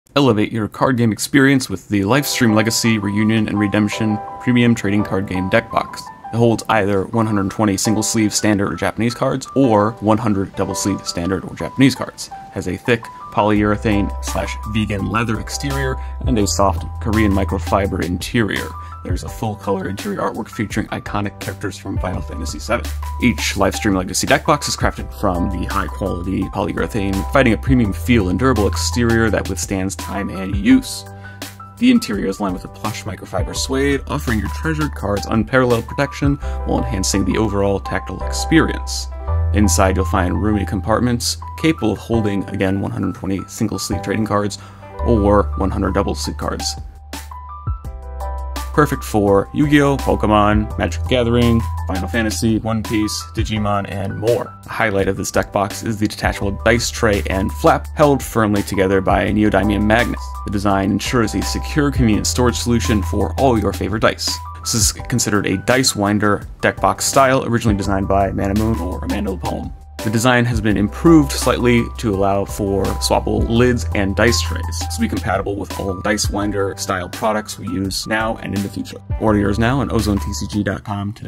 We've Created a Deck Box Series... & More | Lifestream Legacy Series Modular Deck Box | OzoneTCG
Want to pick up cards for an upcoming tournament?

And as always, there'll be more Yu-Gi-Oh content coming soon! We'll update you with more deck boxes and (soon) sleeves!